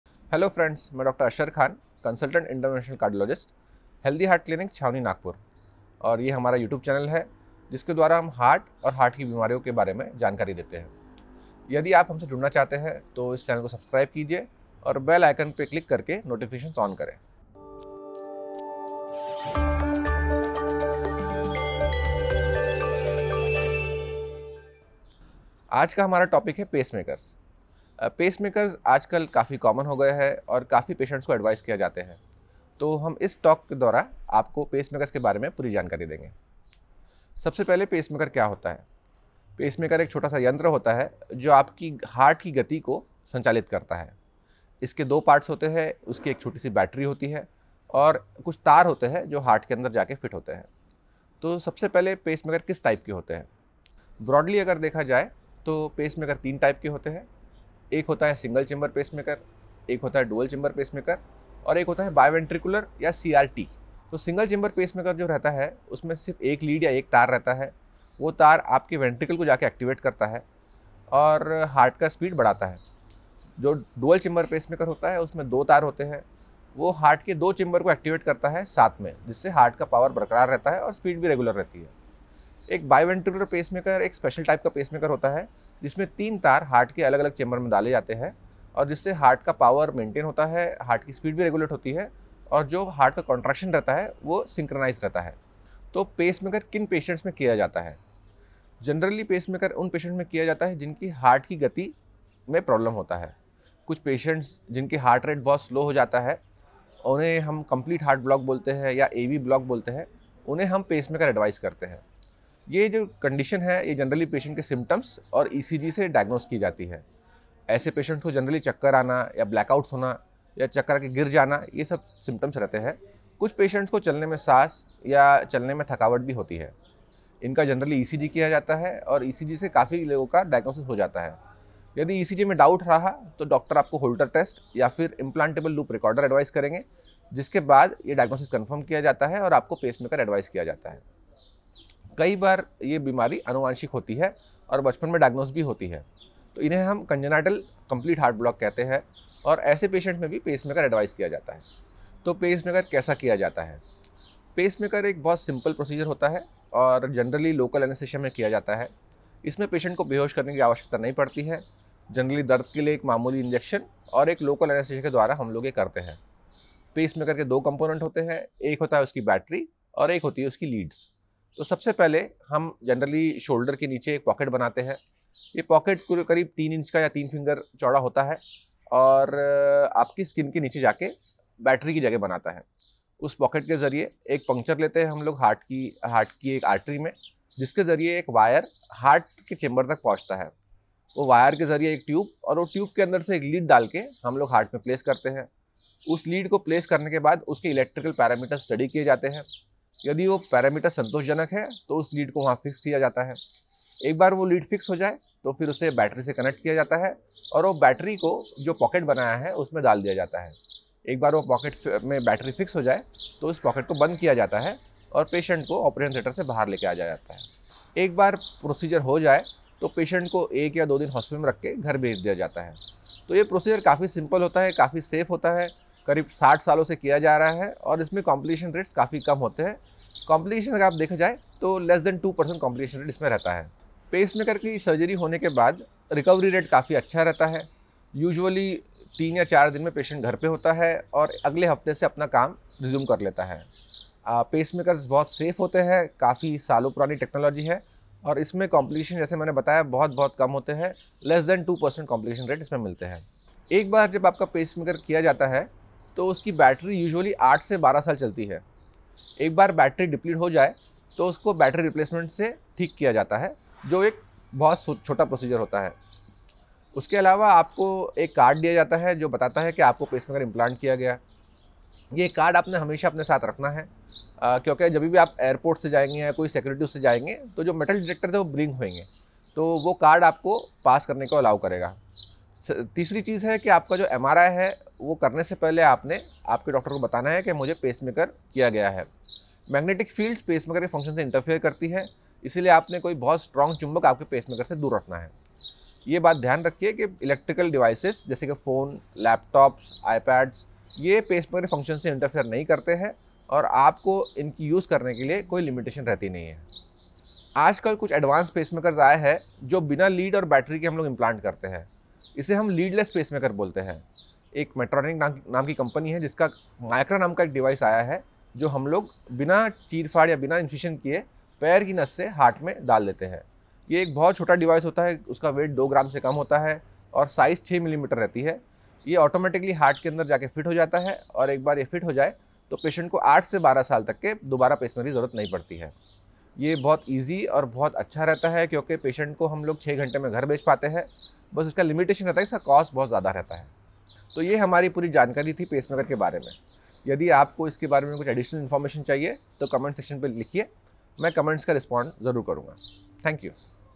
What is Pacemaker? पेसमेकर क्या होता है? जानिए पेसमेकर कैसे काम करता है |Dr Ashar Khan
आज के इस विडियो मे हम जनेगे की पेसमेकर इस यंत्र की मदत से कैसे हार्ट की गति को नियंत्रित किया जाता है, पेसमेकर मानवी हार्ट मे कैसे काम करता है? पेसमेकर किन पेशंट के लिये जरुरी होता है? साथ ही हम जानेंगे पेसमेकर के फायदे और नुकसान के बारे मे।

For any queries or appointments call on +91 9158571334

📍: Healthy Heart Clinic,   Ground Floor, Achraj Tower-2, Besides Balaji Medical,   Chindwara Road, Chhaoni, Nagpur-440013

क्या पेसमेकर के कोई नुकसान होते है.